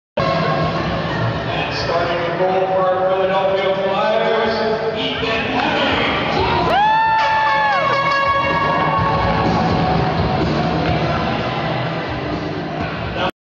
Ethan_light_show.3GP
Ethan Heming at Bulldog Hockey Light Show
Philadelphia Flyers Team